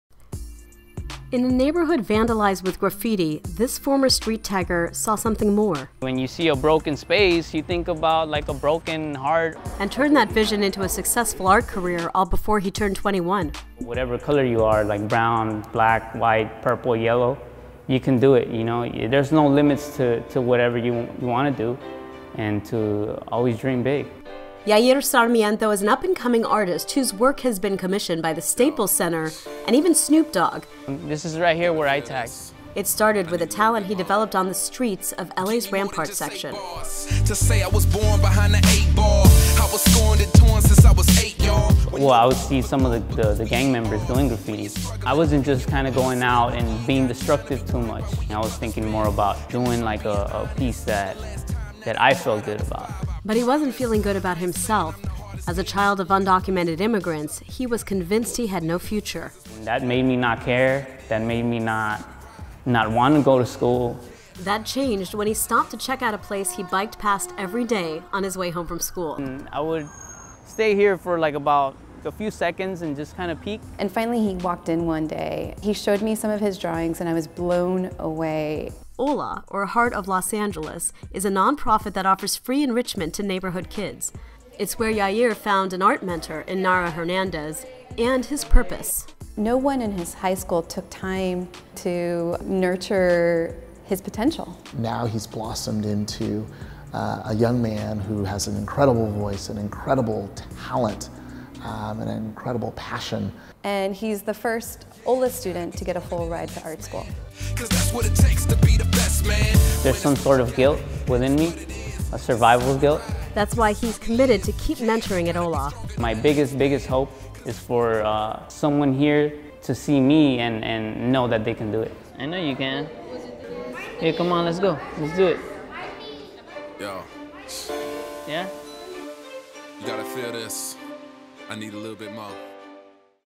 Street Tagger turns into artist and gets a full scholarship to college.
A young man from the Rampart section of LA who was living amongst gang members found his passion in art and got a full scholarship to art school after teachers at HOLA mentored him.